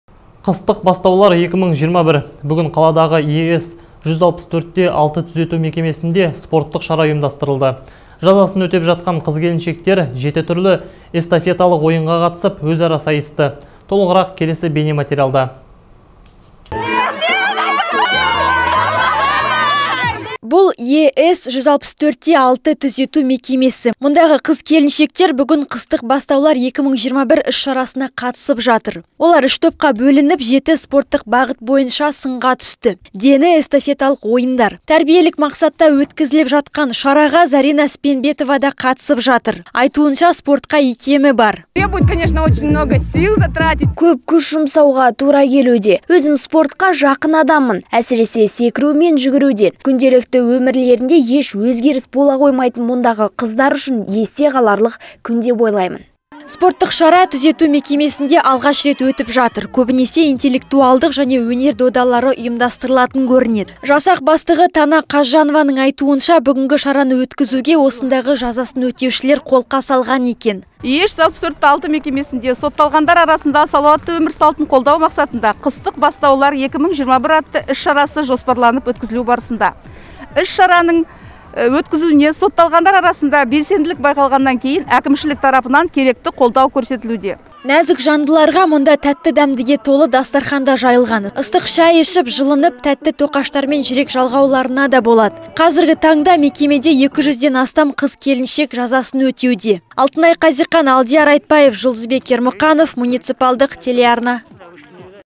ЕС-164/6 мекемесінде спорттық шара ұйымдастырылды
"Қыстық бастаулар-2021". Бүгін қаладағы ЕС-164/6 түзету мекемесінде спорттық шара ұйымдастырылды. Жазасын өтеп жатқан қыз-келіншектер 7 түрлі эстафеталық ойынға қатысып, өзара сайысты. Толығырақ келесі бейнематериалда.